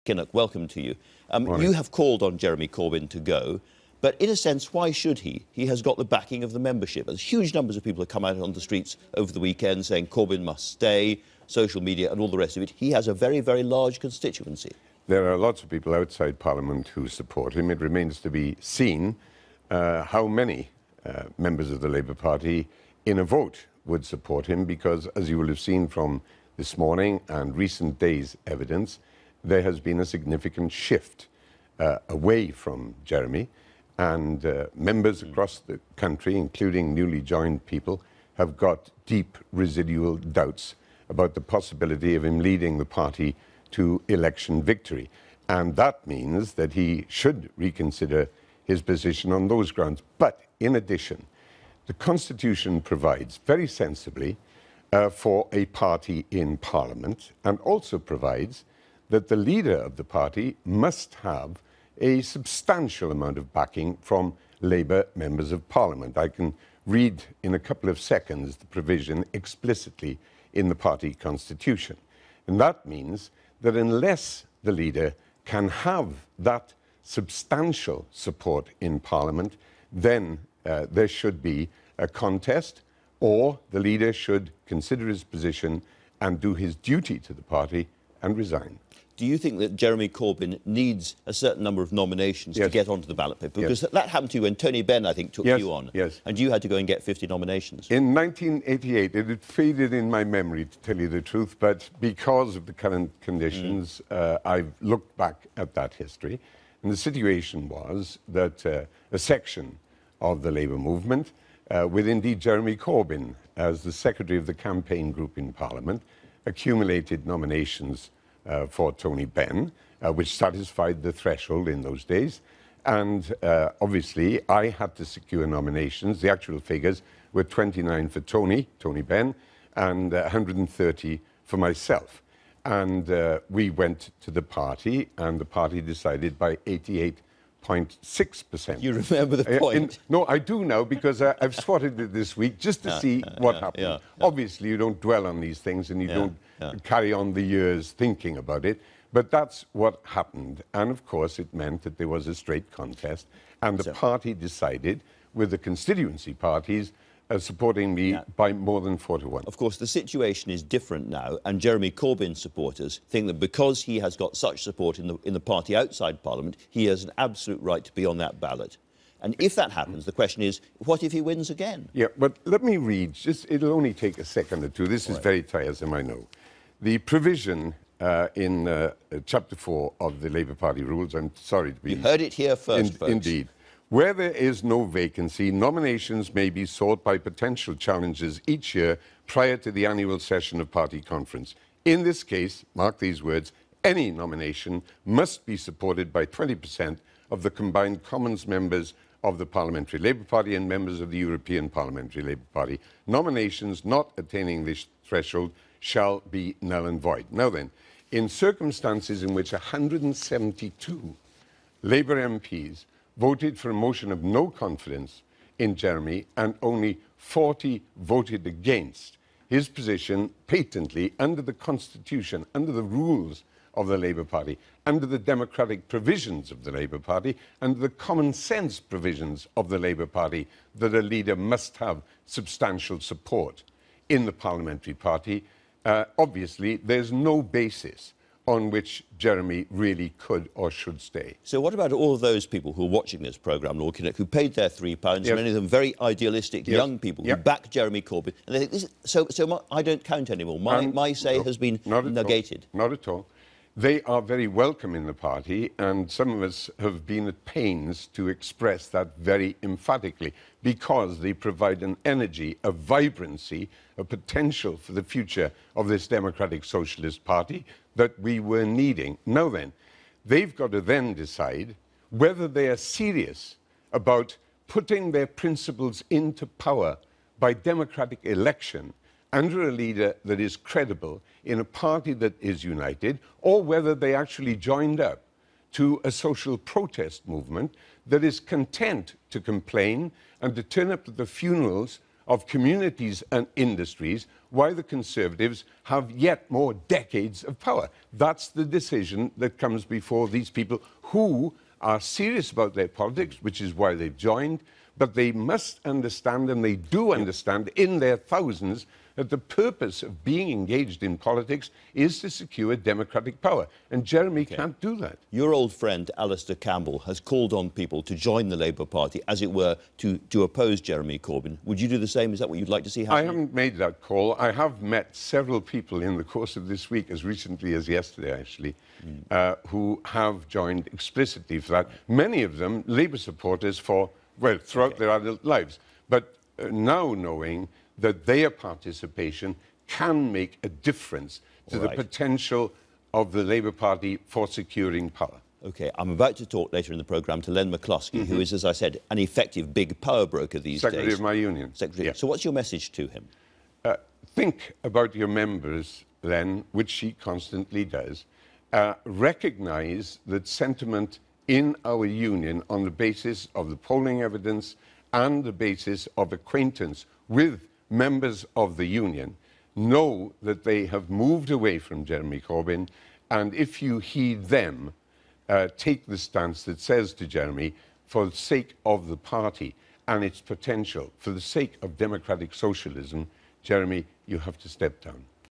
Lord Kinnock: 'Corbyn should do his duty and resign' - BBC News
The Labour leader Lord Kinnock has said there has been "a significant shift" away from supporting Jeremy Corbyn at a grassroots level - and many people joining the party now have "serious doubts" about his suitability for the job. Speaking to Andrew Marr, he read out Chapter 4 of the Labour Party's rules - insisting that Mr Corbyn did not have enough support from parliamentary colleagues to carry on "and should do his duty to the party and resign".